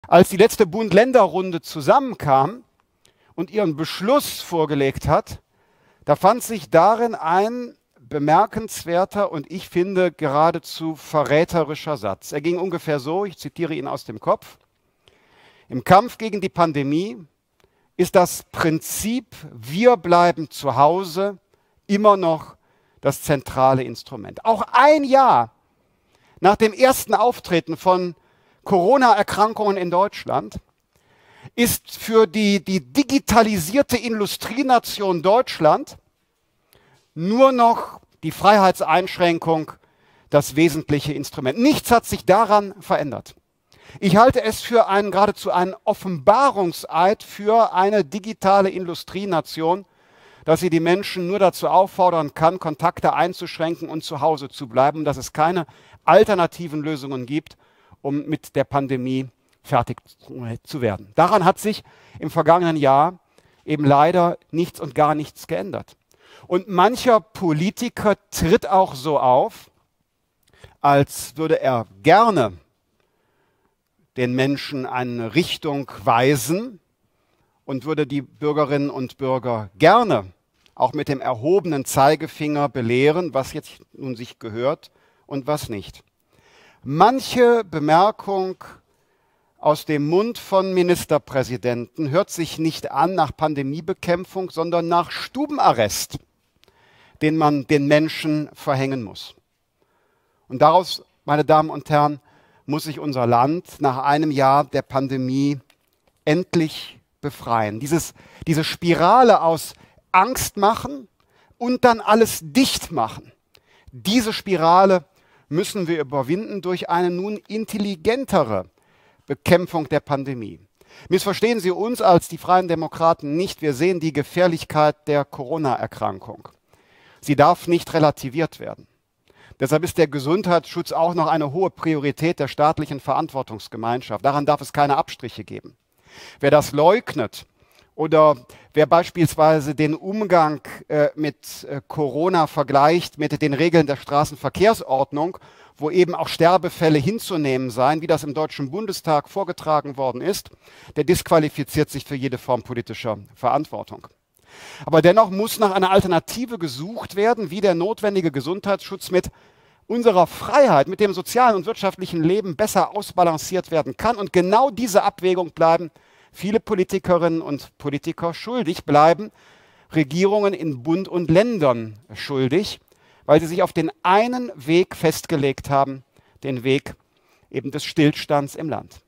Corona intelligenter bekämpfen
Corona ist eine ernst zu nehmende Krankheit und darf nicht relativiert werden. Wir können auch nicht einfach wieder alles aufmachen. Nach einem Jahr Corona kann es aber nicht sein, dass "zu Hause bleiben" die einzige Lösung ist. Wir brauchen eine intelligente Bekämpfung der Pandemie. "Es muss nach einer Alternative gesucht werden, wie der notwendige Gesundheitsschutz mit unserer Freiheit, mit dem sozialen und wirtschaftlichen Leben besser ausbalanciert werden kann. Und genau diese Abwägung bleiben viele Politikerinnen und Politiker schuldig, bleiben Regierungen in Bund und Ländern schuldig, weil sie sich auf den einen Weg festgelegt haben, den Weg des Stillstands im Land", so Christian Lindner.